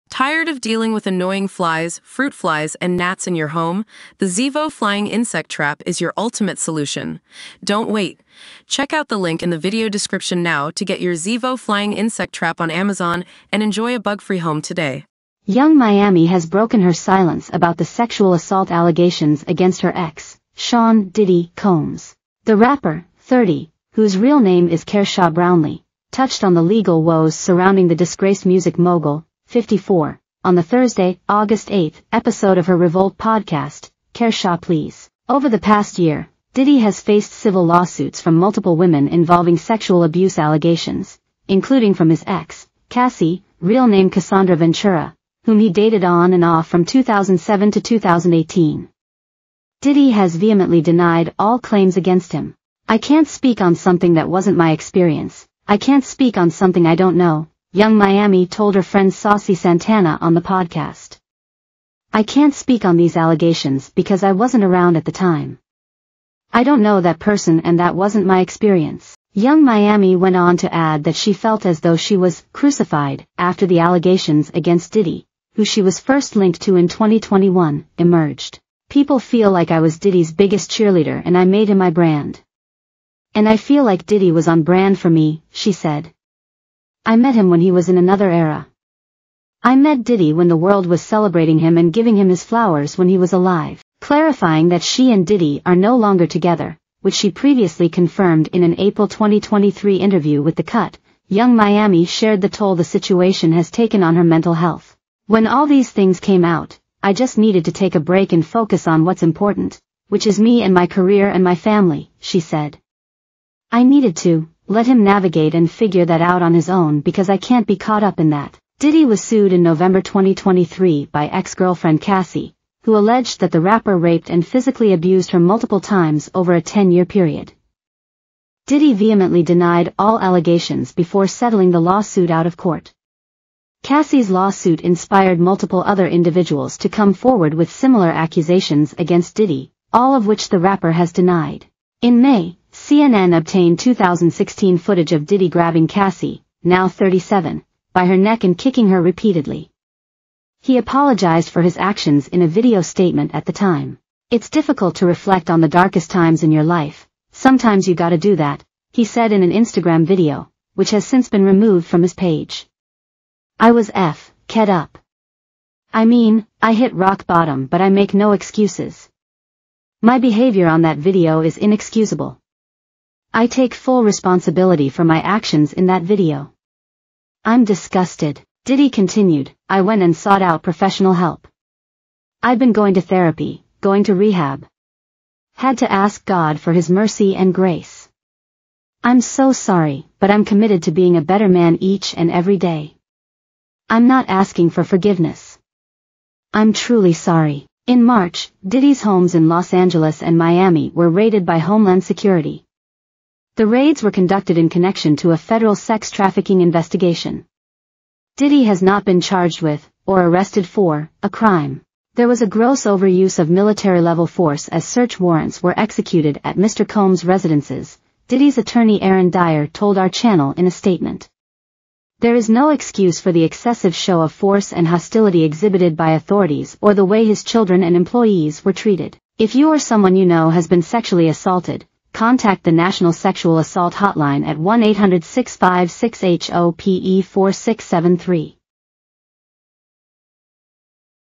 Yung Miami explains why she didn't speak out about Diddy's sexual assault allegations: "That wasn't
Lila Moss sets pulses racing in a tiny black two-piece swimsuit as she soaks up the sun in Formentera with her mother Kate and boyfriend Yoni Helbitz.